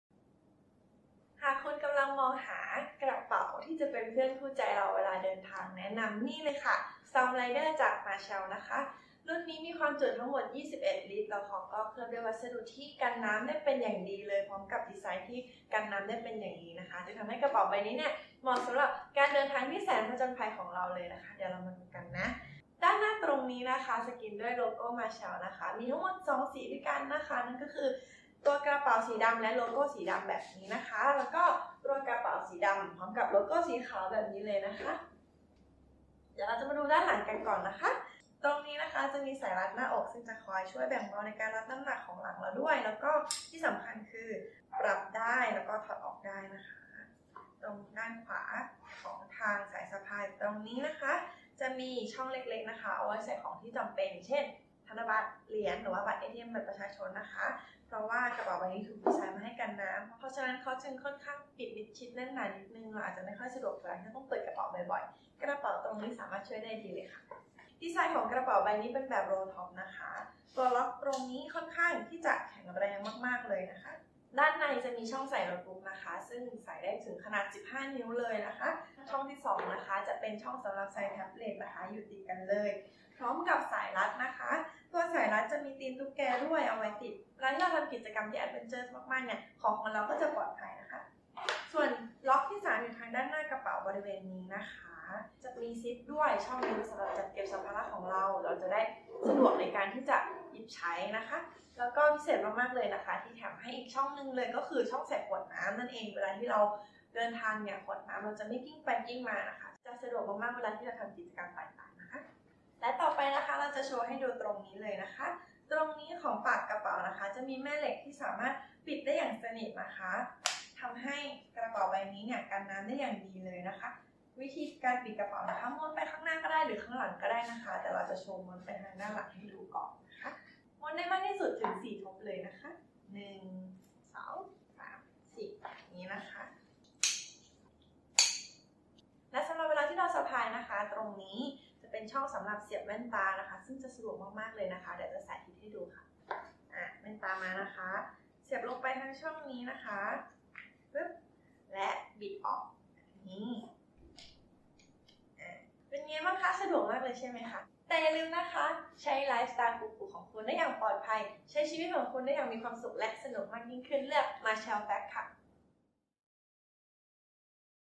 รีวิวกระเป๋า Marshall - Storm Rider Backpack
ทำให้ของมีค่าของคุณไม่เปียกชื้น พร้อมท่องเที่ยวไปกับกระเป๋ากันน้ำขนาด 20 ลิตร ผลิตจากวัสดุที่ทนทาน และเคลือบด้วยPVC พร้อมกับสายรัดหน้าอก ที่สามารถถอดออกได้ เหมาะสำหรับนักเดินทาง ที่ชอบผจญภัยไปกับสิ่งที่ชอบ ท่องเที่ยวไปยังสถานที่ต่างๆที่มีความเปียกชื้น

ลักษณะเฉพาะ:
วัสดุของกระเป๋าทำจาก PET 1680D ที่ทนทานและแข็งแรง
ที่จับซิป ทรงกระบอก ปั๊มโลโก้ Marshall
ซิปกันน้ำที่กระเป๋าด้านข้าง
ตัวล็อคกระเป๋าแข็งแรง ปรับระดับได้ สะดวกและทนทานต่อการใช้งาน
ด้านบนของกระเป๋าดีไซน์แบบ Rolltop
ด้านในกระเป๋าเลือกใช้สีที่แตกต่างกันอย่างลงตัว
ช่องเสียบแว่นตาอยู่ด้านนอกกระเป๋า เพื่อความสะดวกในการใช้งาน
มีช่องใส่แล็ปท็อป
หูหิ้วกระเป๋าด้านบนทำจากไมโครไฟเบอร์
ขนาด:
ขนาด(บรรจุสัมภาระแล้ว): 43 x 34 x 10ซม.
ขนาด(ก่อนบรรจุสัมภาระ): 53 x 39 x 5 ซม.
ความจุ: 20 ลิตร
น้ำหนัก: 0.52กก. (520ก.)
ความสามารถในการบรรจุ:
MacBook Pro 15นิ้ว หรือน้อยกว่า
iPad ที่มีขนาด 9 นิ้วหรือน้อยกว่า